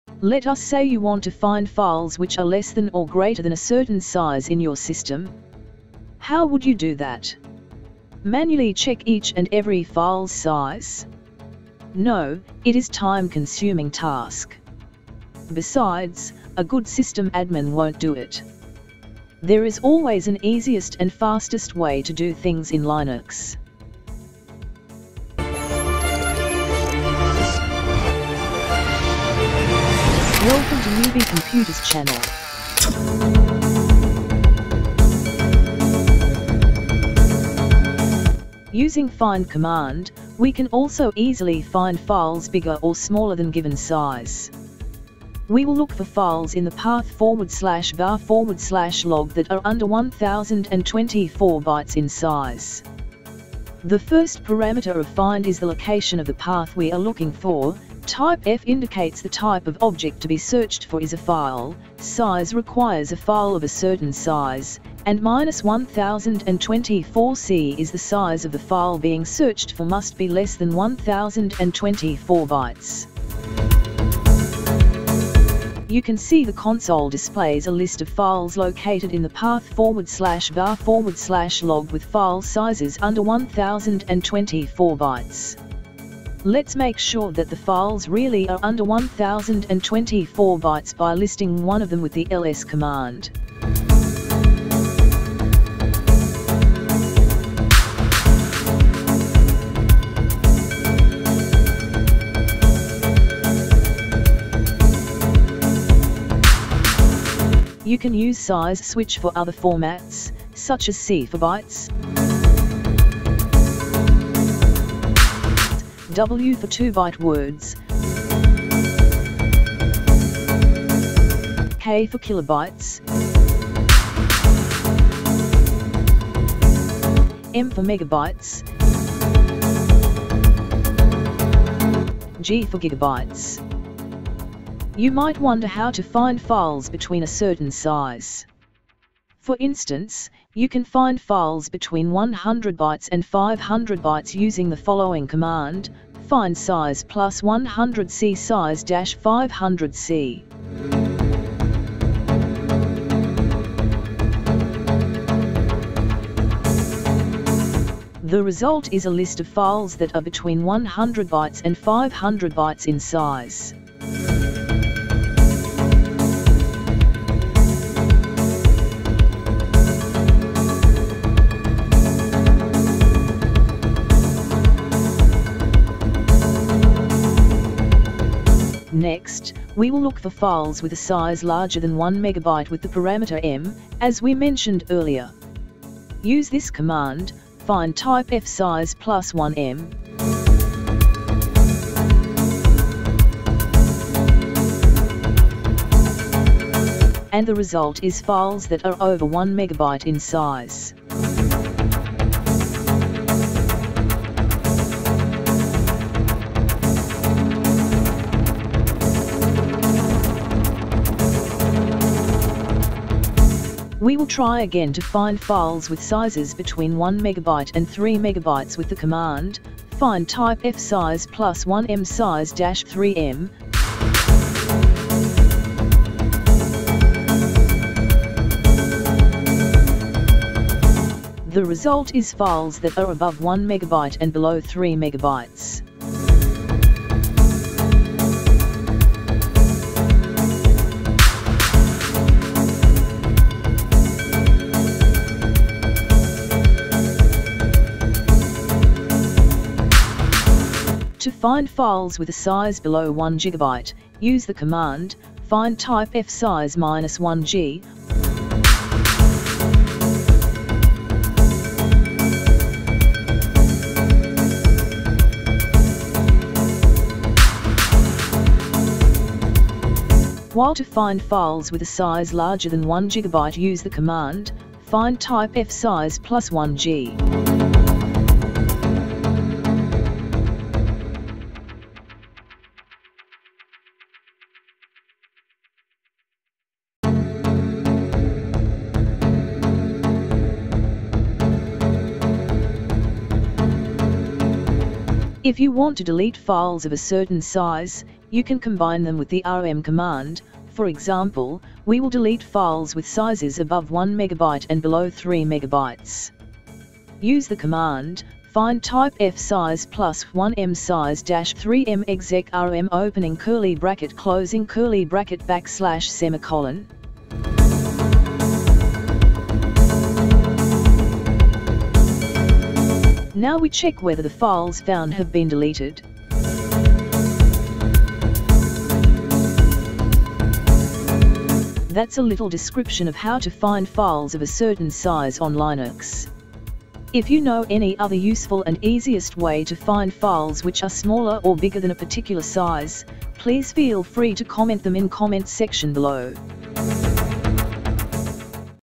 How To Find/delete Files Bigger Or Smaller Than X Size In Linux bash command easy tutorial
Let us say you want to find files which are less than or greater than a certain size in your system. How would  you do that? Manually check each and every file's size? No, it is time consuming task. Besides, a good system admin won't do it. There is always an easiest and fastest way to do things in Linux. This brief tutorial covers how to find files bigger or smaller than X size in Linux and Unix operating systems.

if you want to delete files of a certain size,  for example, files with sizes above 1 megabyte and below 3 megabytes. use the command: find -type f -size +1M -size -3M -exec rm {} \;